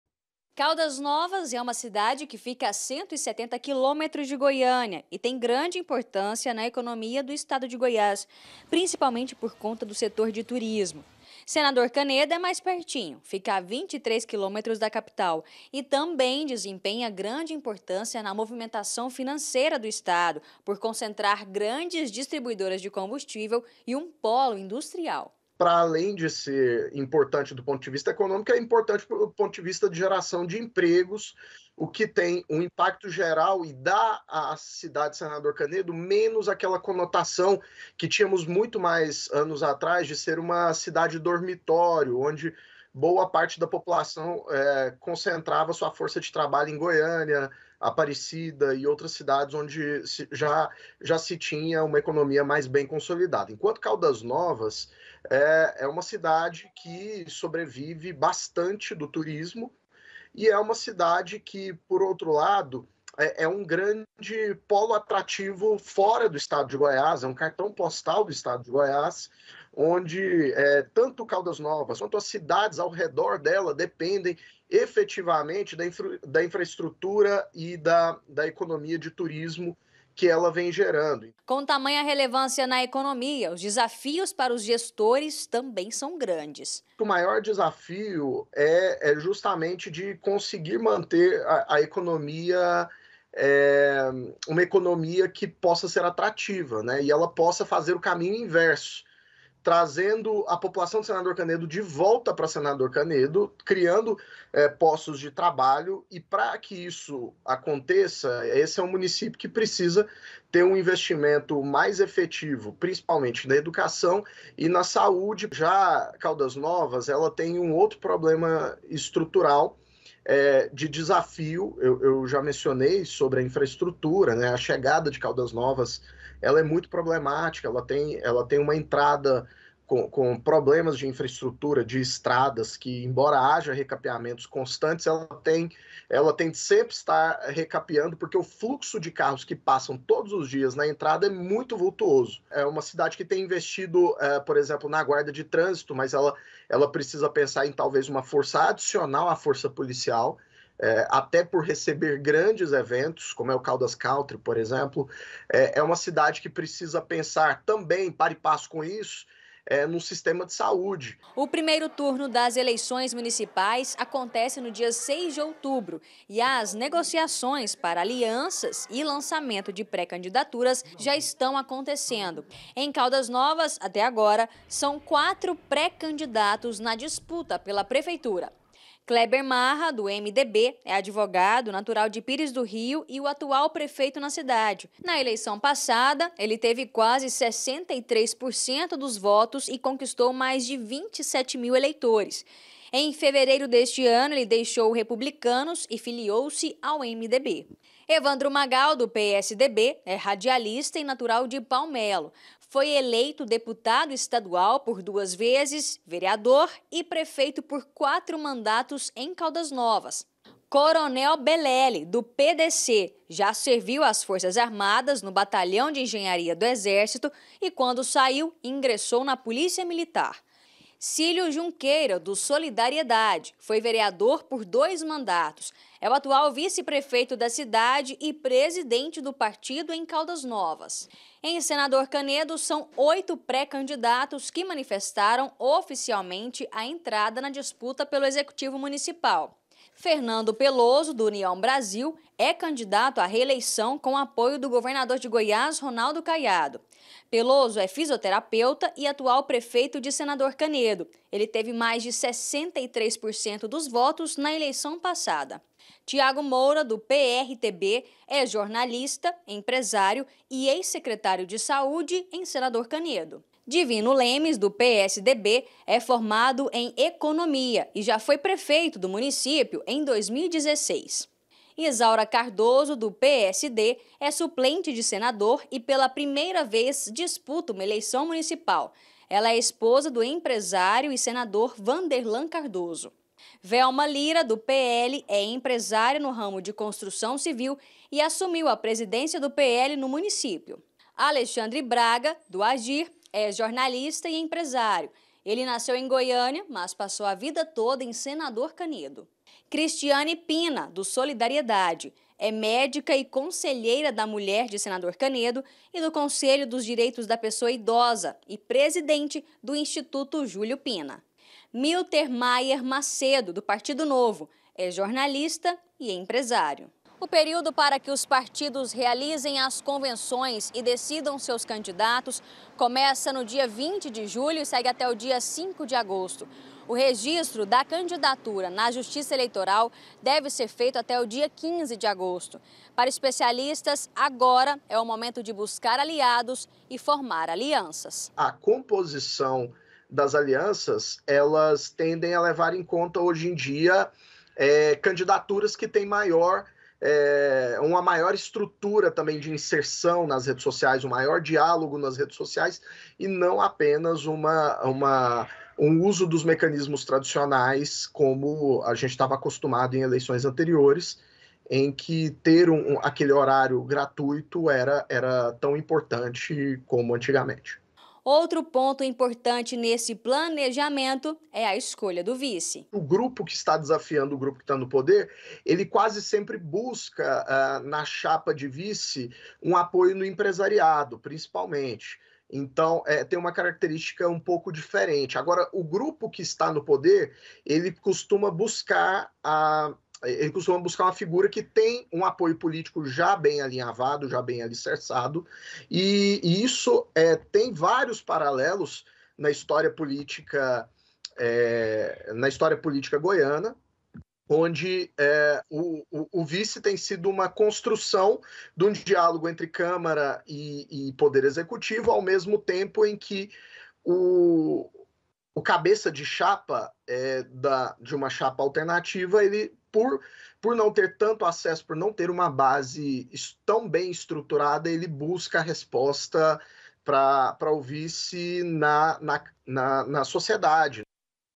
ELEIÇÕES 2024: DESAFIOS DE SENADOR CANEDO E CALDAS NOVAS
SENADOR CANEDO E CALDAS NOVAS SÃO DUAS DAS CIDADES MAIS IMPORTANTES DE GOIÁS. UMA COMO POLO TURÍSTICO E A OUTRA COMO PÓLO DE LOGÍSTICA. E AS DUAS COM  GRANDES ORÇAMENTOS. A DISPUTA PELA PREFEITURA DEVE SER MUITO ACIRRADA.